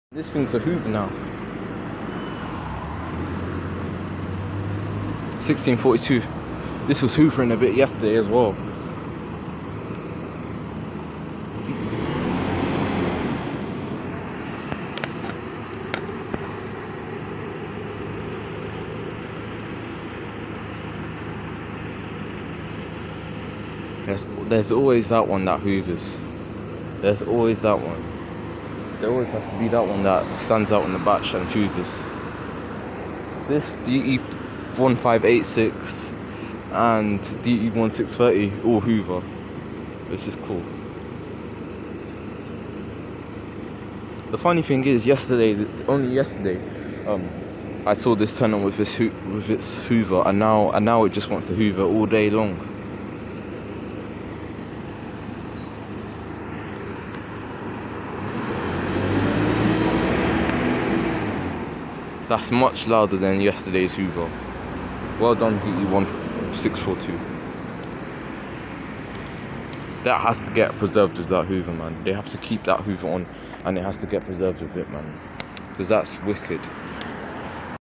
COOLING FAN | ML DE1642 On Bus Route 226 (Spot)
As said in the video..

I saw this yesterday at the exact moment it's cooling fan switched on but today its alot louder which is cool, I might record it if I'm bothered.